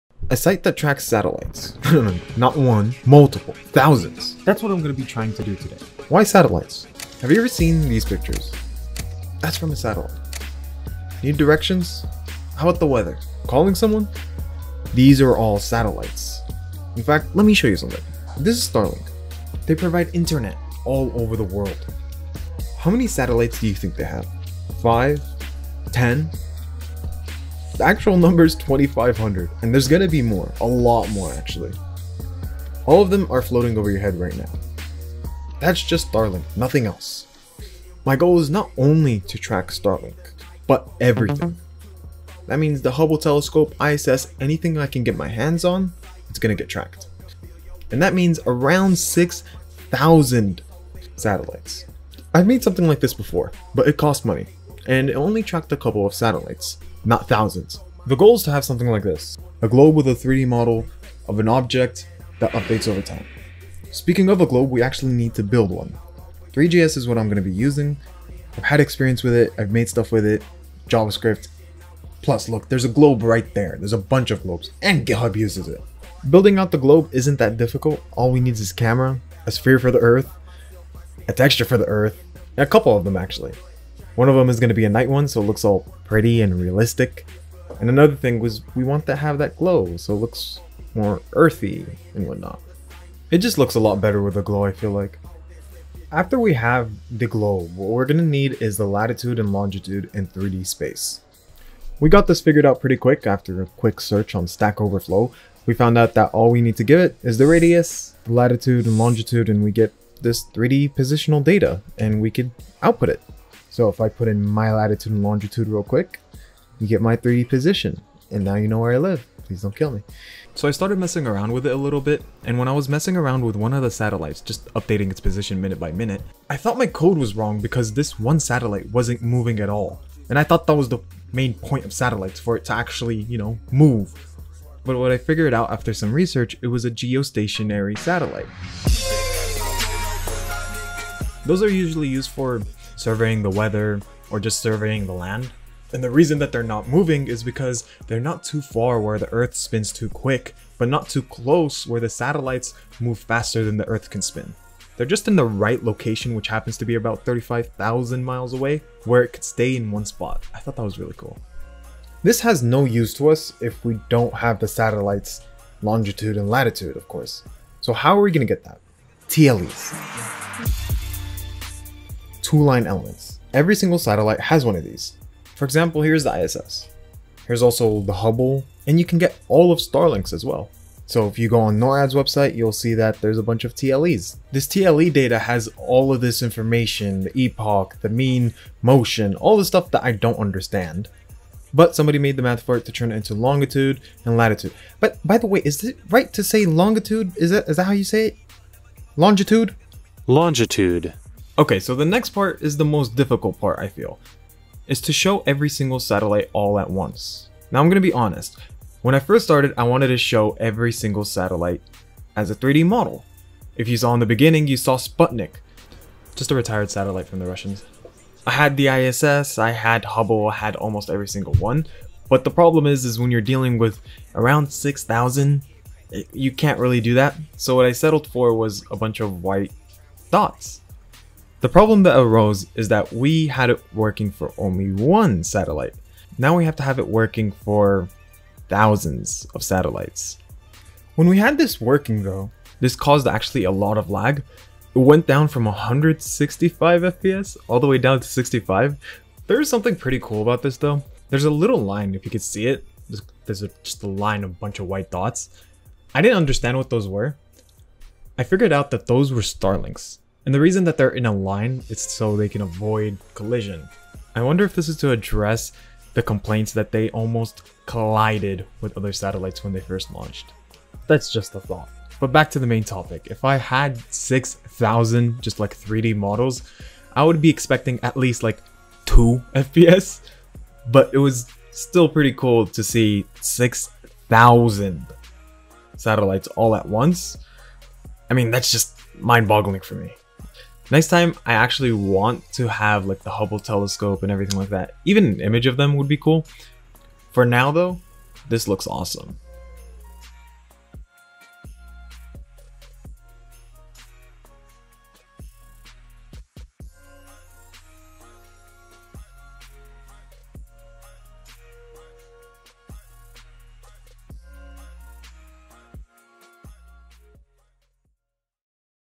Creating A Site To Track EVERY Satellite Ever Made | JavaScript Node JS
Creating a Site to Track EVERY Satellite EVER. This was Programmed and Created in Javascript using NODE JS and THREE JS.  The most notable Satellites are Starlinks, Hubble, and the ISS. It tracks 6500 Satellites and all of their Lat Long comes from TLES. This came straight from Norad. Hopefully next time I can create a way to have 3d models or some images.